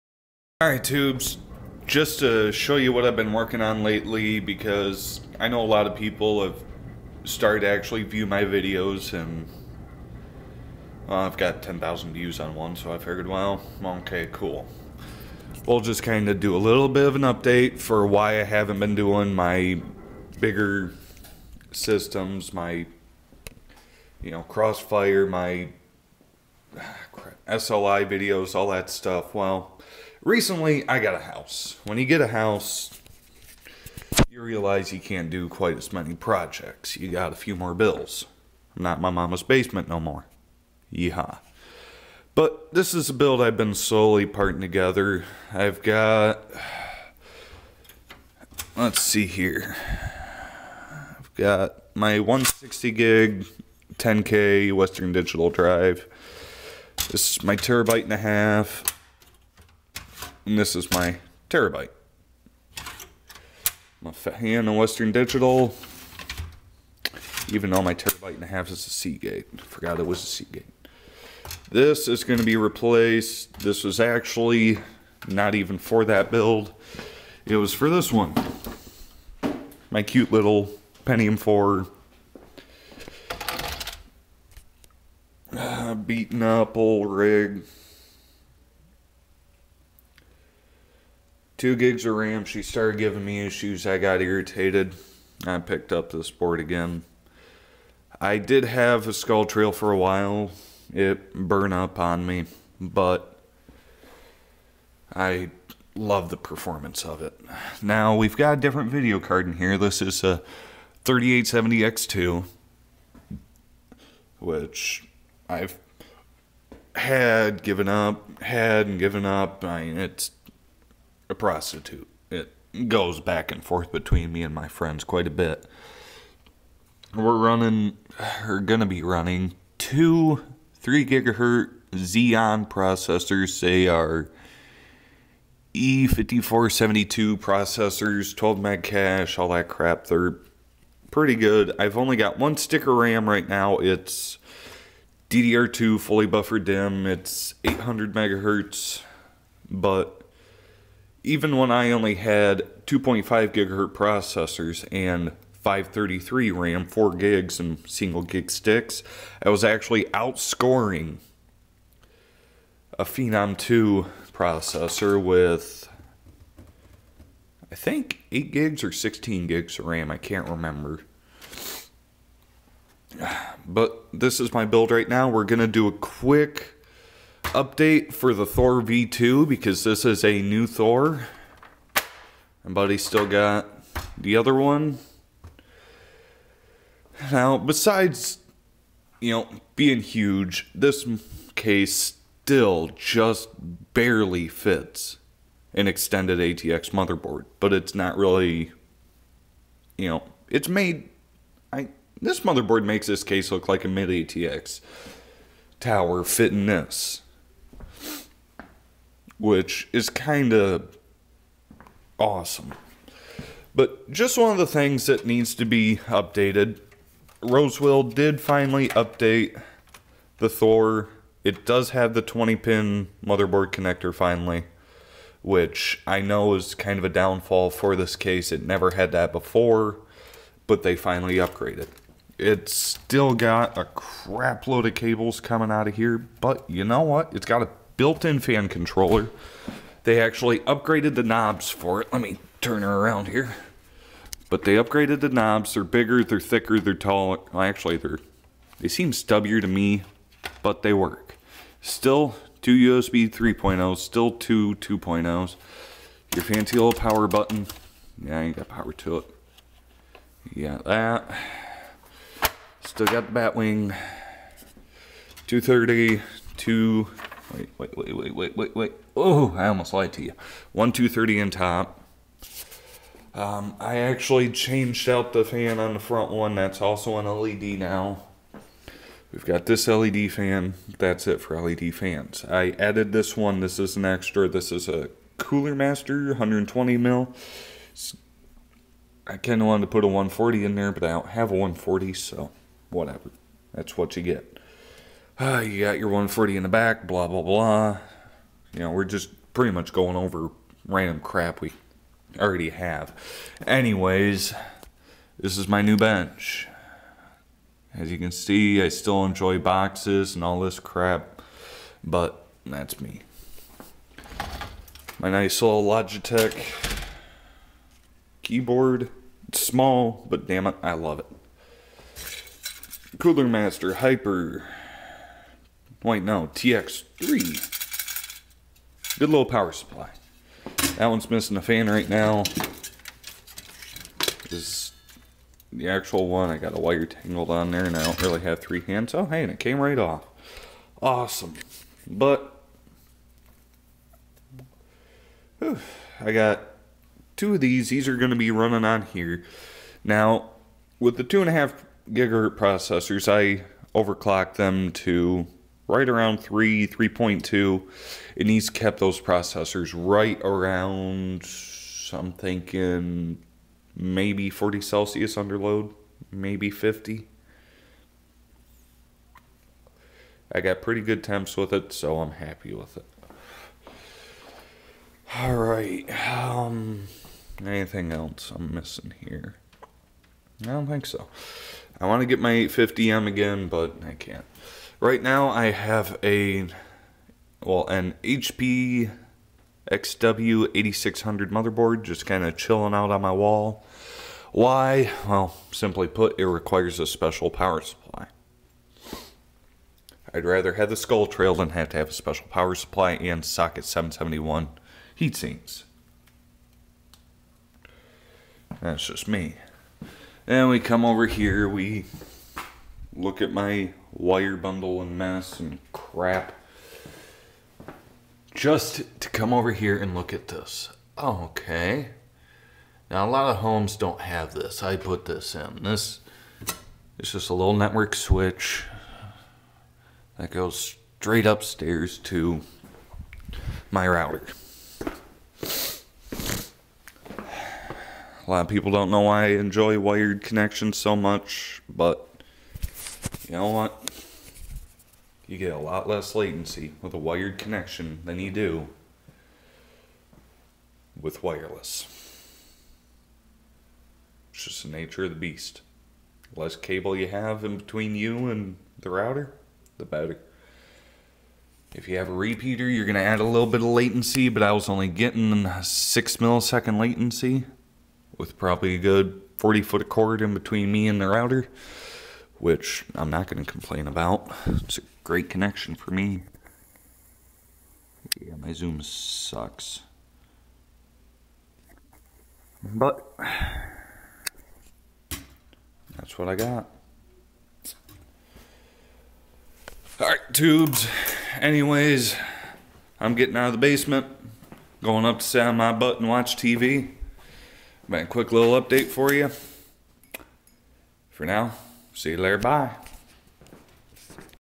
new intel build and whats up with r3d
im getting ready for building season and the season looks good with prices for components staying at reasonable amounts and technology improving every day. 22nm process has been achieved now its improving motherboard and gpu bandwidths.
motherboard is an intel d5400xs with two e5472 intel xeon cpu's (771 socket) two coolermaster tx3 heatsinks 2gb 800mhz fbdimm memory. ati 3870x2 gpu.